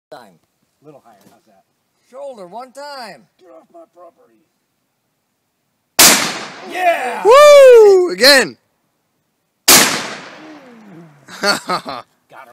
win106
106 year old Winchester double barrel shotgun ...from the hip.